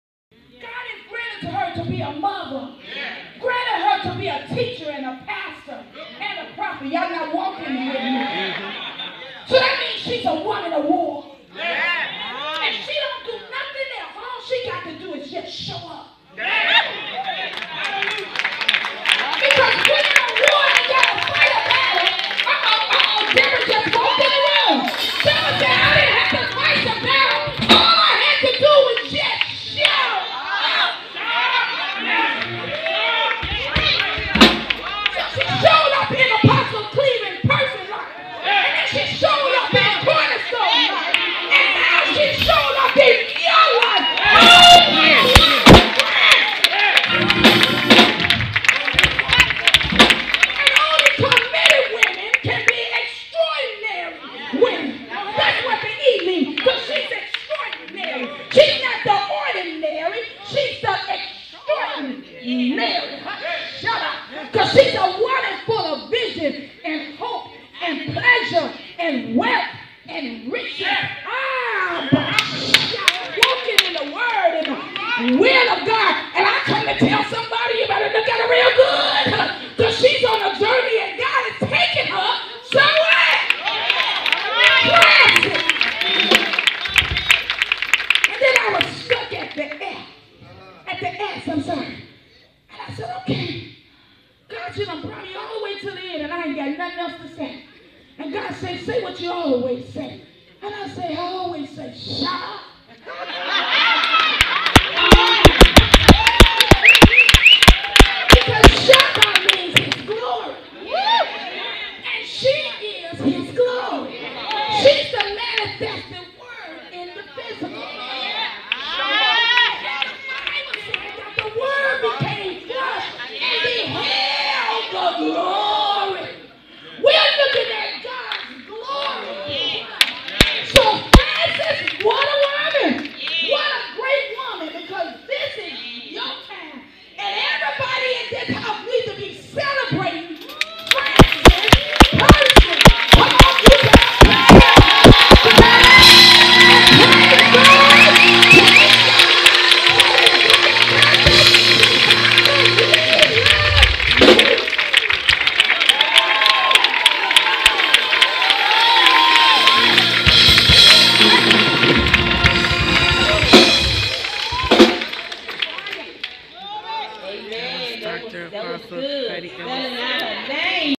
CORNER STONE CHURCH OF CHICAGO
Apostle Heidi Ellis speaking a word over Apostle Frances L. Person.